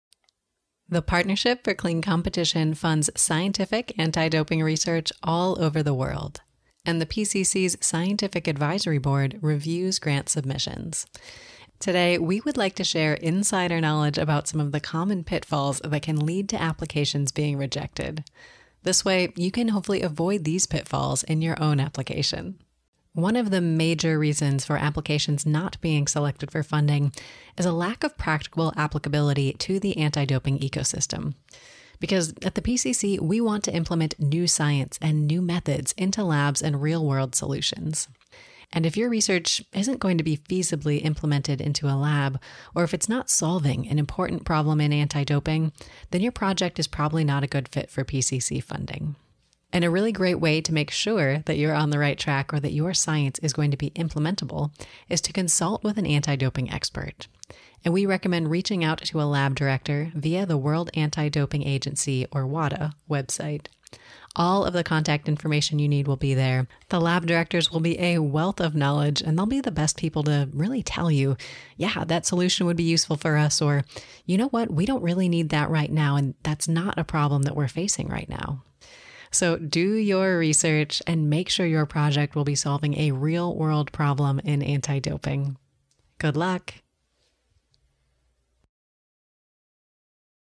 Common Pitfalls in PCC Grants - #2
We're sharing insider knowledge about some of the common pitfalls that can lead to PCC Research Grant applications being rejected. This video focuses on potential impacts in the field of anti-doping.